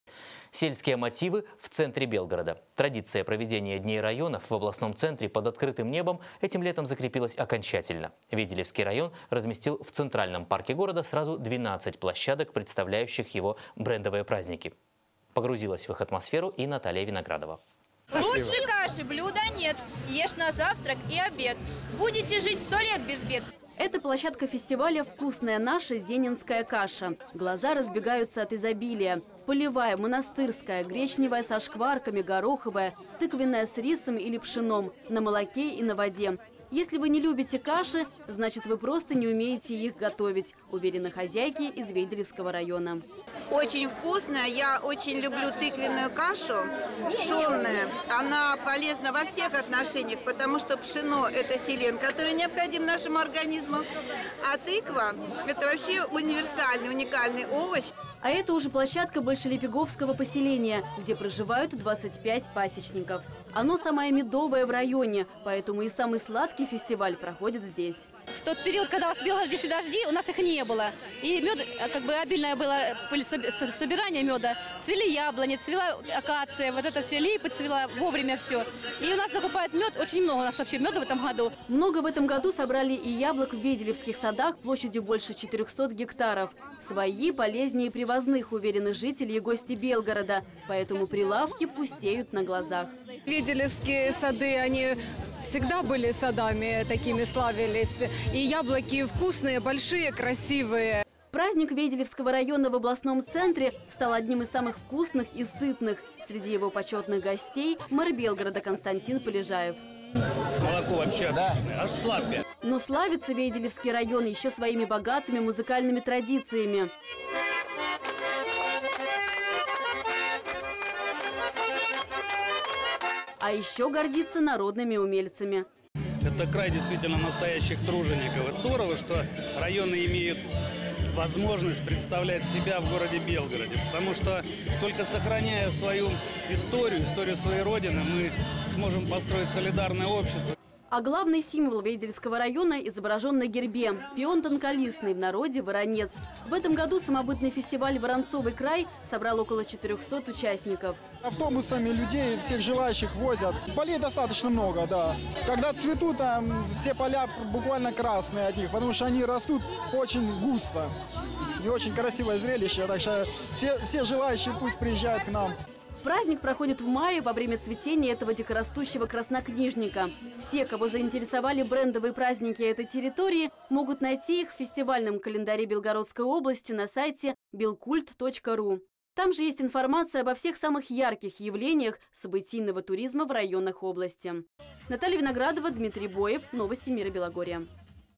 День Вейделевского района в Белгороде
Сельские мотивы в центре Белгорода. Традиция проведения дней районов в областном центре под открытым небом этим летом закрепилась окончательно. Вейделевский район разместил в Центральном парке города сразу 12 площадок, представляющих местные брендовые праздники. Погрузилась в их атмосферу и Наталья Виноградова.